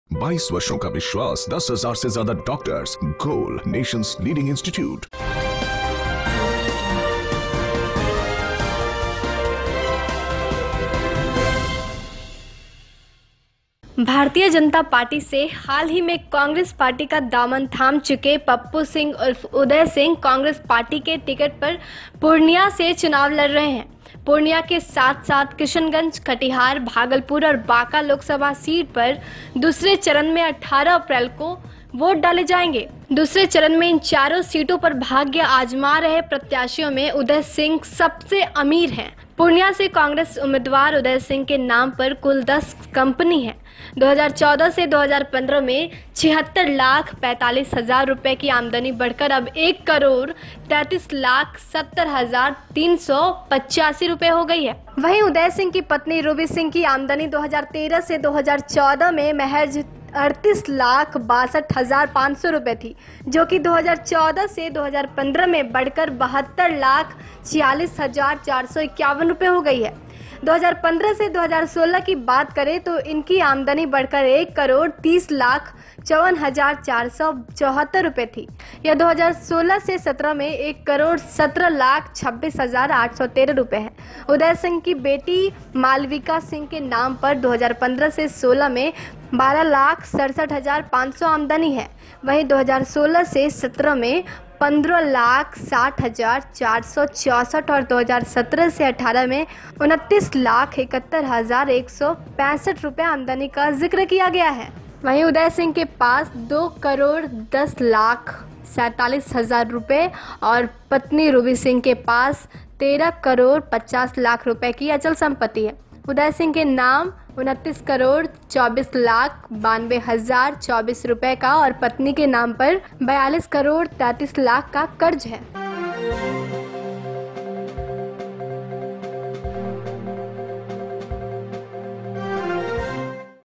सबसे अमीर हैं पूर्णिया के पूर्व सांसद pappu singh
city post live special - सबसे अमीर हैं पूर्णिया  के पूर्व सांसद pappu singh |